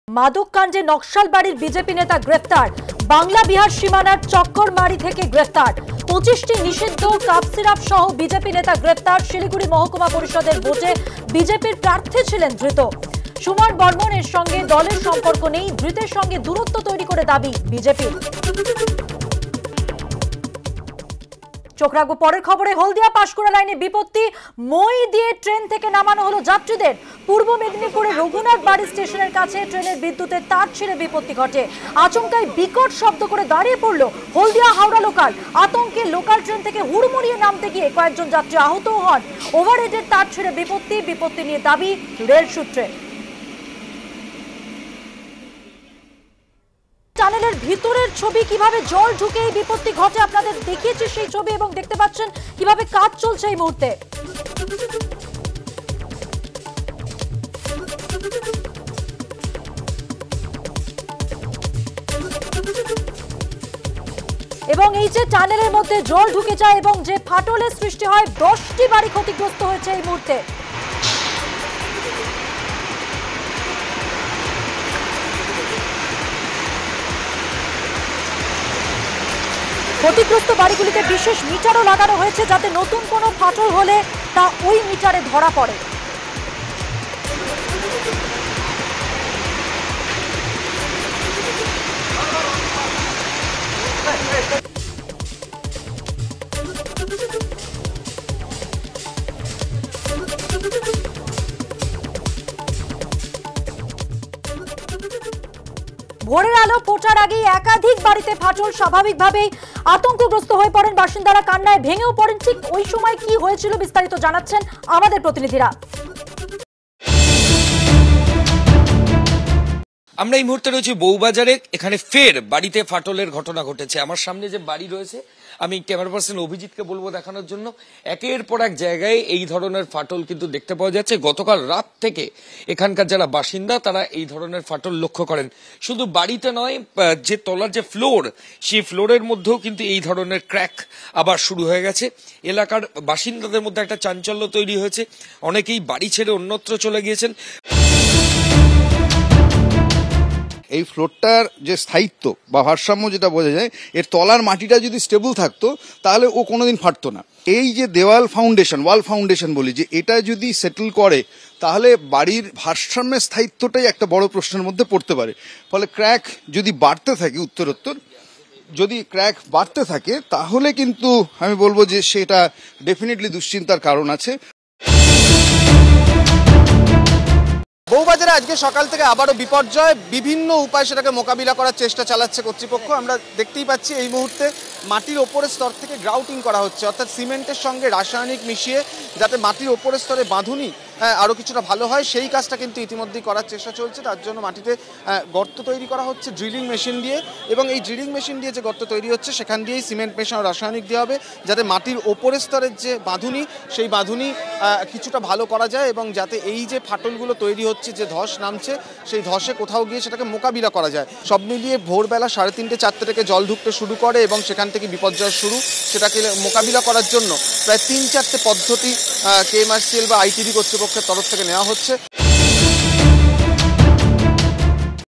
BJP Leader Arrest: মাদককাণ্ডে নকশালবাড়ির বিজেপি নেতা গ্রেফতার
মাদককাণ্ডে নকশালবাড়ির বিজেপি নেতা গ্রেফতার। বাংলা-বিহার সীমানার চক্করমাড়ি থেকে গ্রেফতার। ২৫টি নিষিদ্ধ কাফ সিরাপ-সহ বিজেপি নেতা গ্রেফতার শিলিগুড়ি মহকুমা পরিষদের ভোটে বিজেপির প্রার্থী ছিলেন ধৃত সুমন বর্মন। এর সঙ্গে দলের সম্পর্ক নেই, ধৃতের সঙ্গে দূরত্ব তৈরি করে দাবি বিজেপির